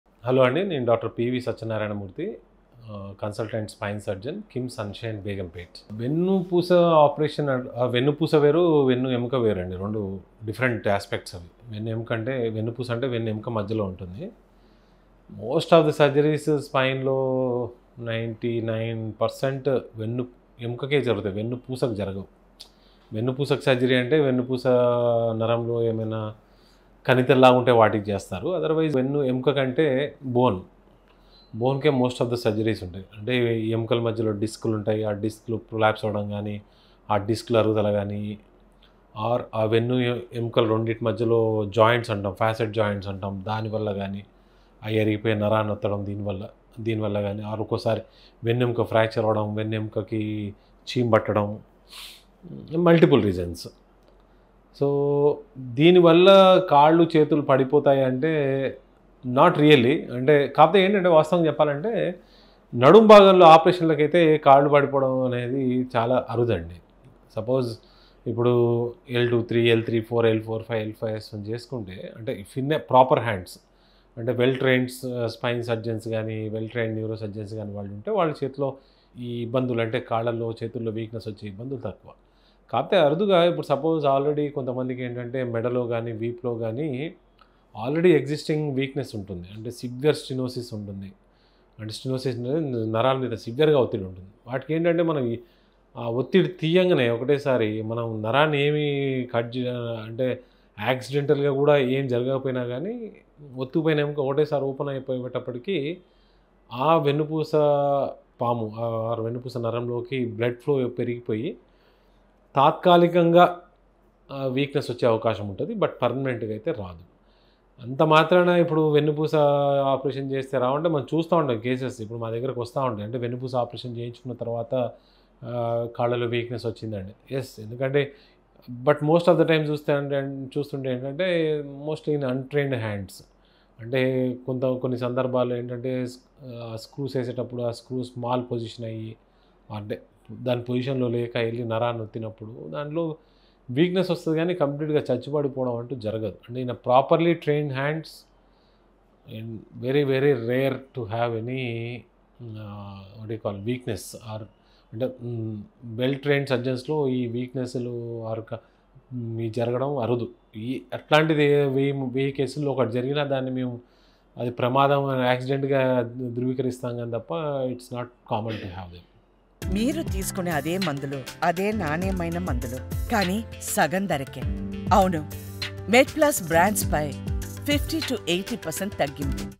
Post-Spinal Surgery: Addressing Weakness or Paralysis in Hands and Legs | Dr.PV Satyanarayana Murthy
After spinal cord surgery, some patients may experience weakness or loss of function in their legs and hands. Unfortunately, there is often no clear medical explanation for this issue